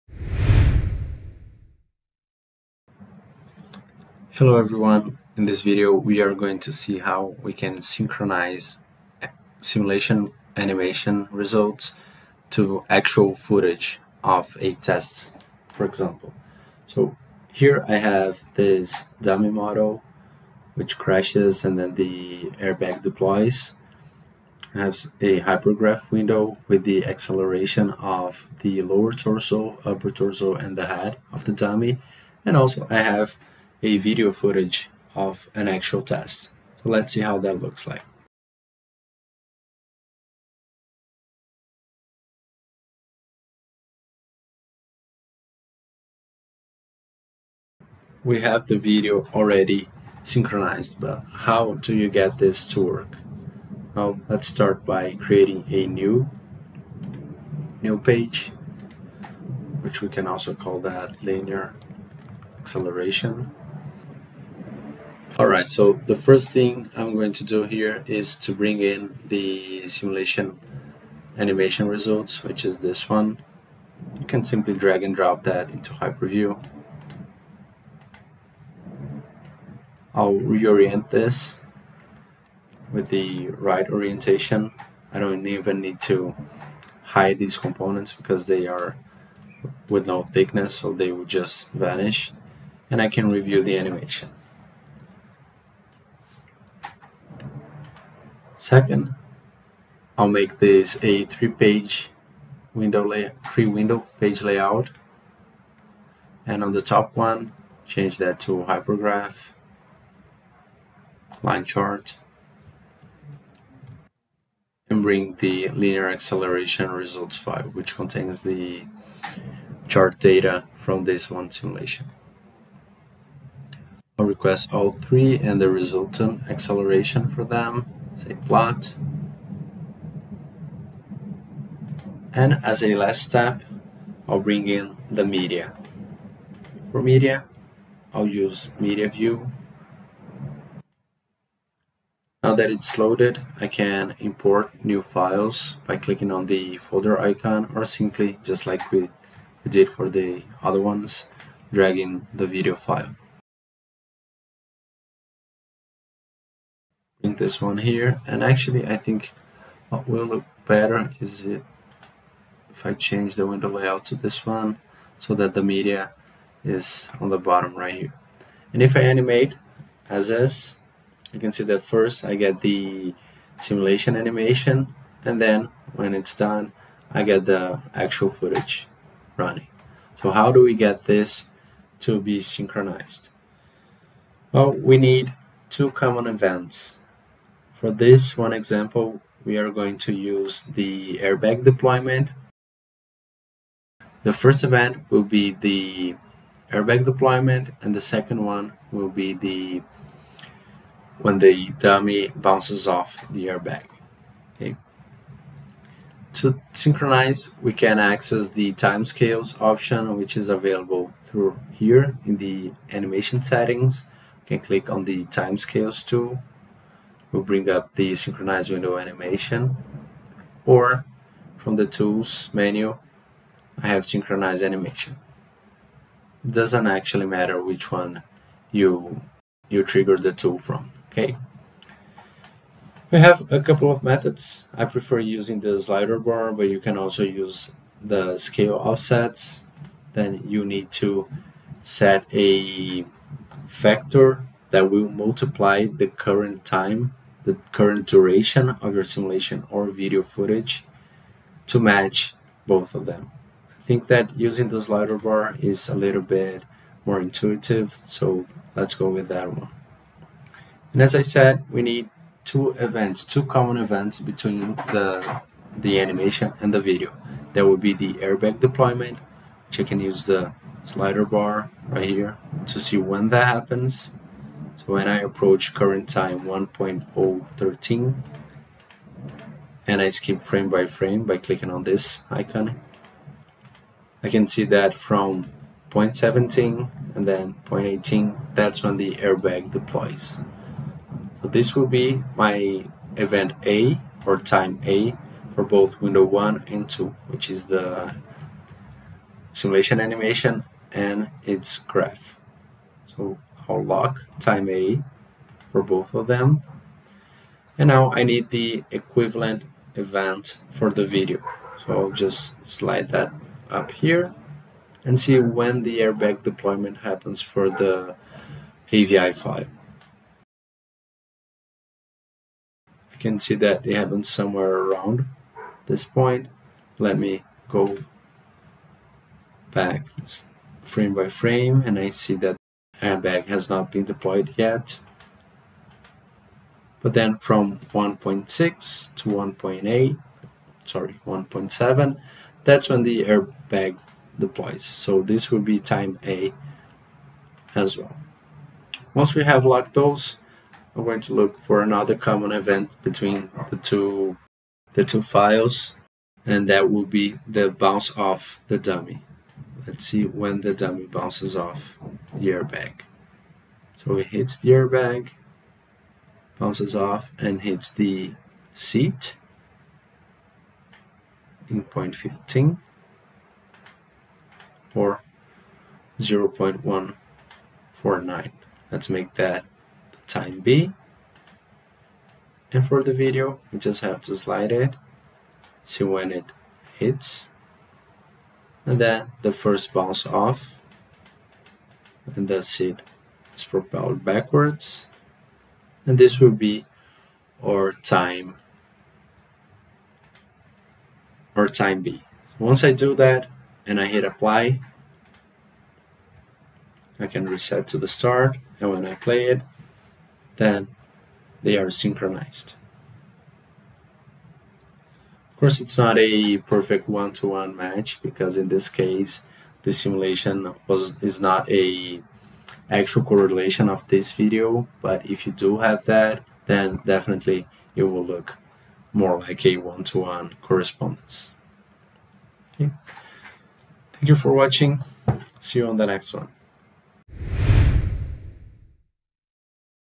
How to Synchronize Animation Results with Video
Discover how to use the Synchronize Animation tool in Altair HyperView with this step-by-step video tutorial. This post-processing feature allows you to synchronize animations, videos and charts across multiple windows, enabling a comprehensive analysis of simulations. Learn to compare different results side-by-side and enhance your engineering insights.